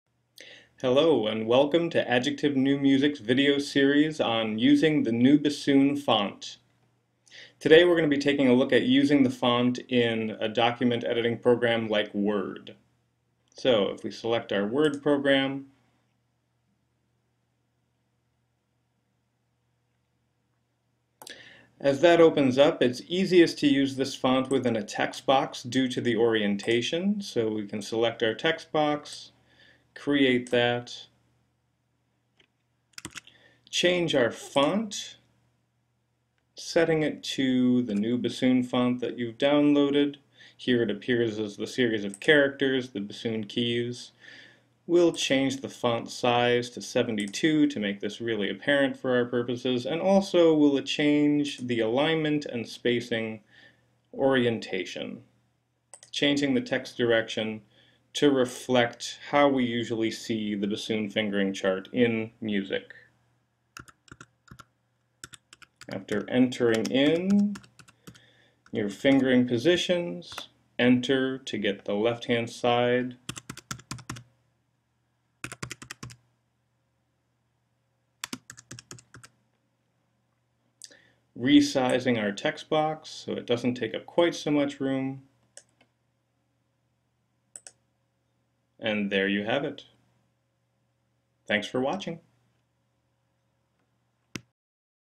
USING THE NEUE BASSOON FONT IN WORD
A basic guide to using the Neue Bassoon font in MS Word.